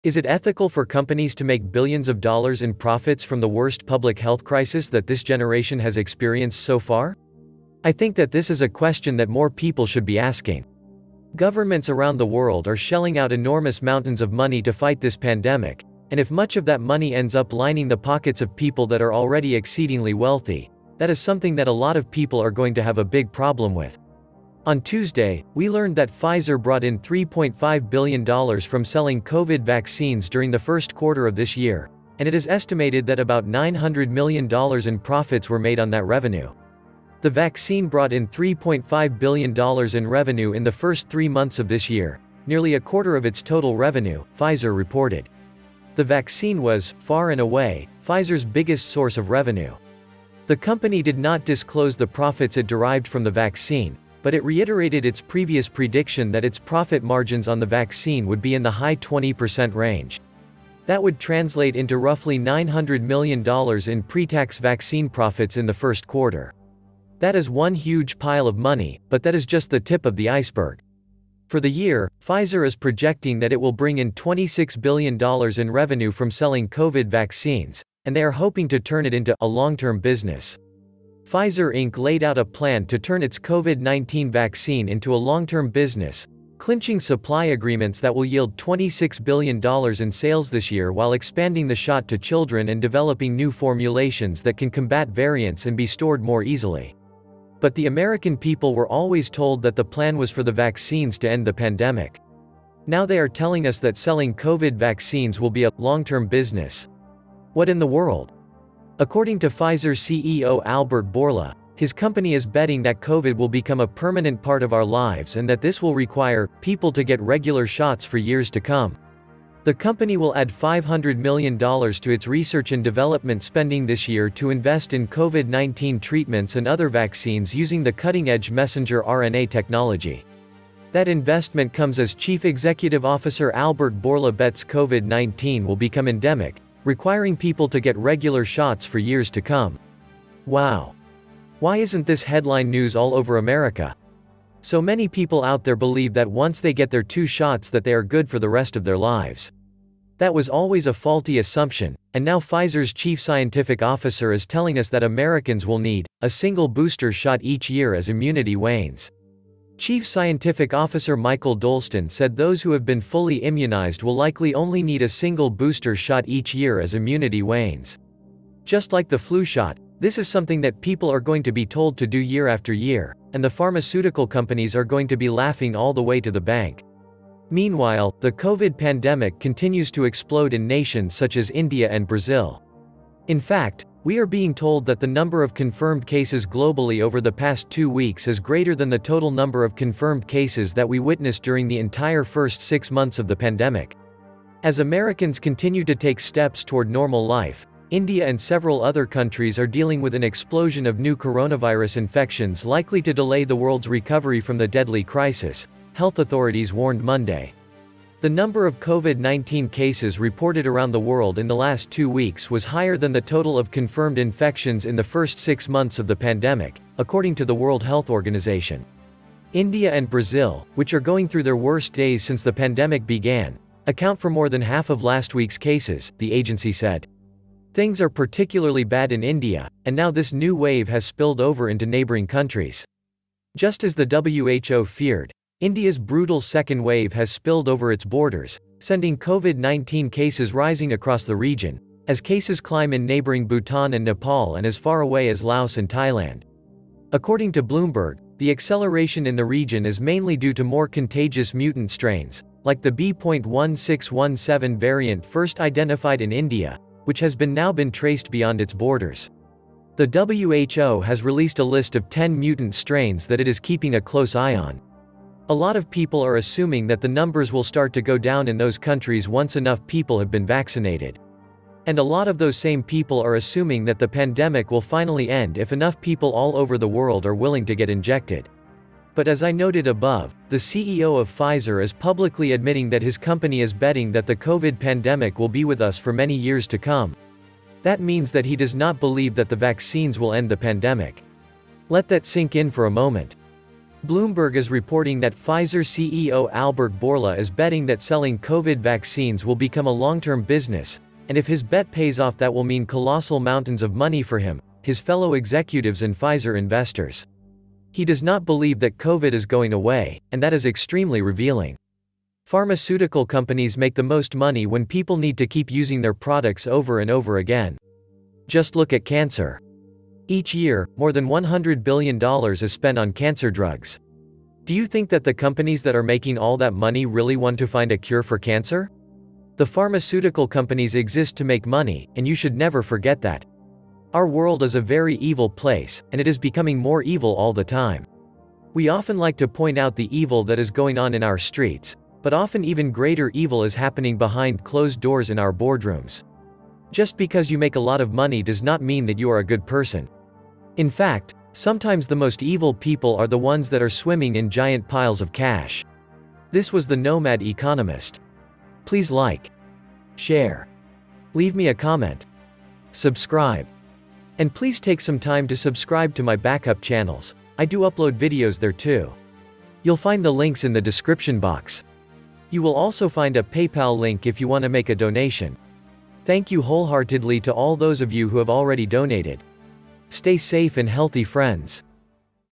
Big Pharma Laughing All The Way to The Bank thanks to The Pandemic
Is it ethical for companies to make billions of dollars in profits from the worst public health crisis that this generation has experienced so far?  I think that this is a question that more people should be asking.  Governments around the world are shelling out enormous mountains of money to fight this pandemic, and if much of that money ends up lining the pockets of people that are already exceedingly wealthy, that is something that a lot of people are going to have a big problem with.  On Tuesday, we learned that Pfizer brought in 3.5 billion dollars from selling COVID vaccines during the first quarter of this year, and it is estimated that about 900 million dollars in profits were made on that revenue…

The vaccine brought in $3.5 billion in revenue in the first three months of this year, nearly a quarter of its total revenue, Pfizer reported. The vaccine was, far and away, Pfizer’s biggest source of revenue.